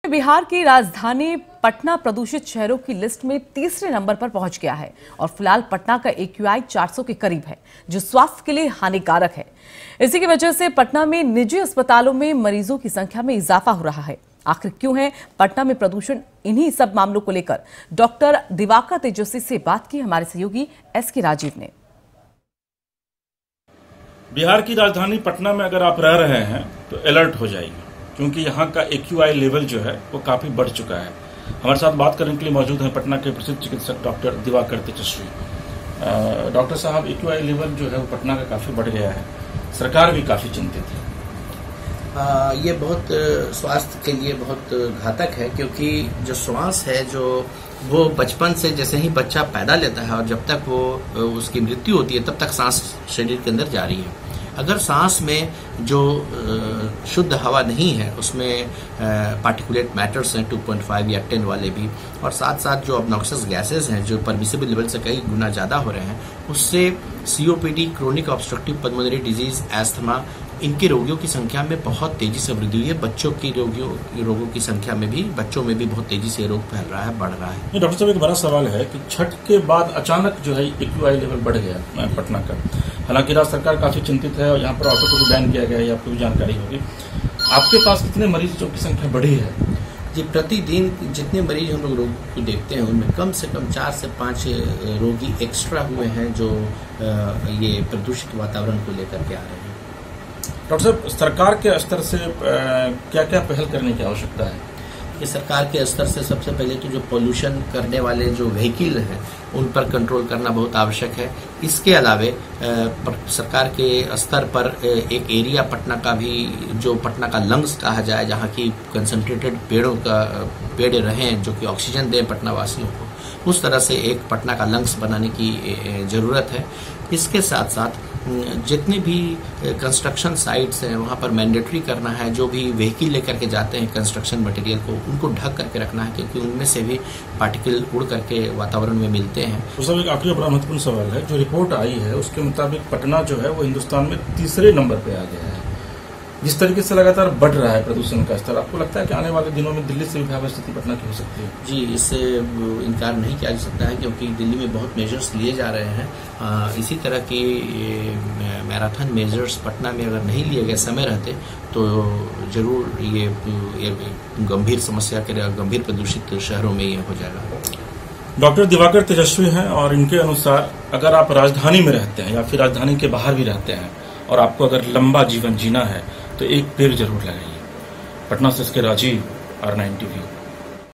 प्रदूषित शहरों में Patna तीसरे नंबर पर पहुंचा
देश में बिहार की राजधानी पटना प्रदूषित शहरों की लिस्ट में तीसरे नंबर पर पहुंच गया है और फिलहाल पटना का AQI 400 के करीब है जिसकी वजह से पटना में निजी अस्पतालों में मरीजों की संख्या में ईजाफा हो रहा है...